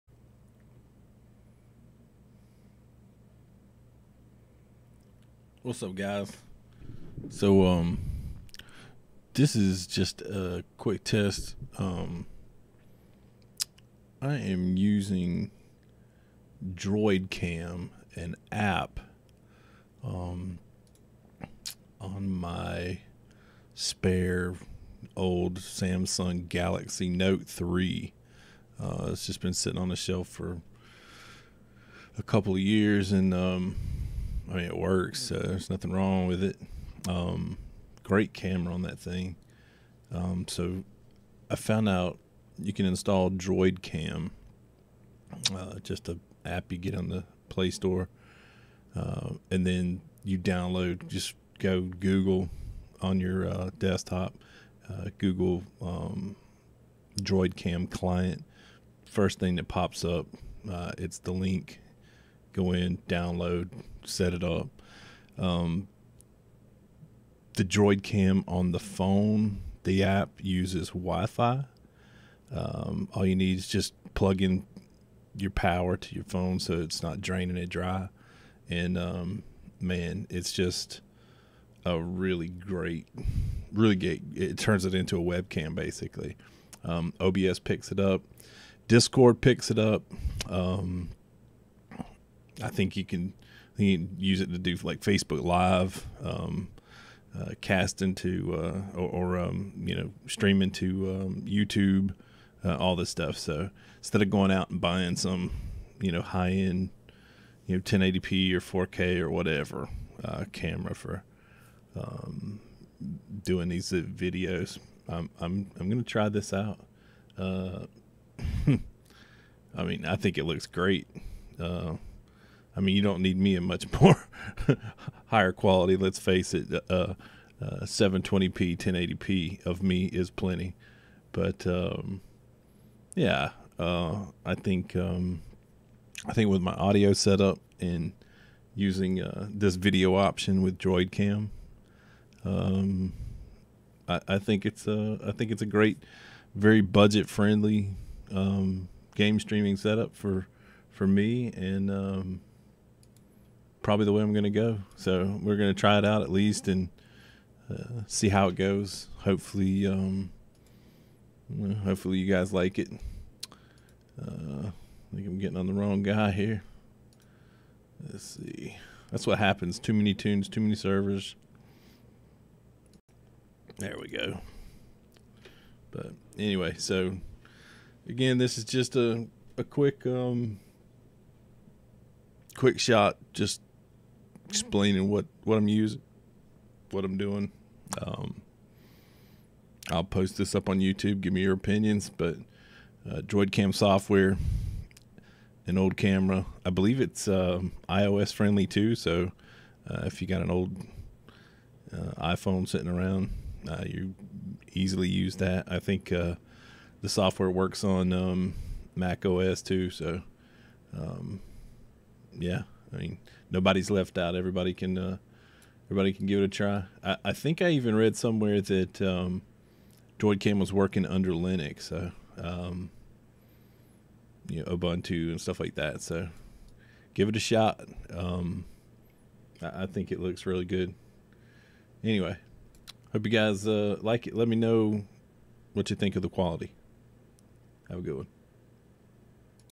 Using DroidCam on old Android phone as a webcam for live streaming & recording game play
Test using DroidCam as a webcam with OBS Studio and streaming or recording game play. Seems to work great let me know your thoughts.